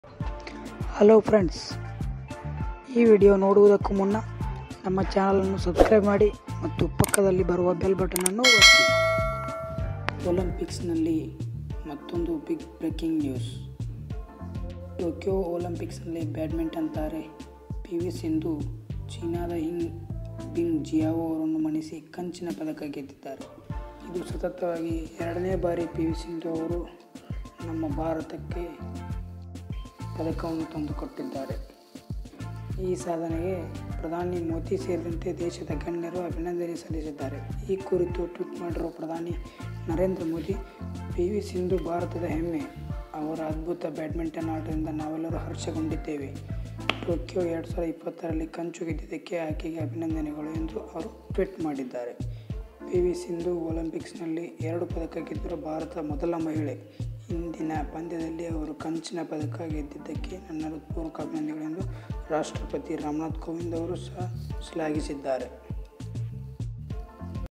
ಭಾರತಕ್ಕೆ ಕಂಚಿನ ಪದಕವನ್ನು ತಂದುಕೊಟ್ಟ ಪಿ.ವಿ ಸಿಂಧು||Tokiyo Olympics||PV Sindhu
Top all in one YouTube▶️ channel in karnataka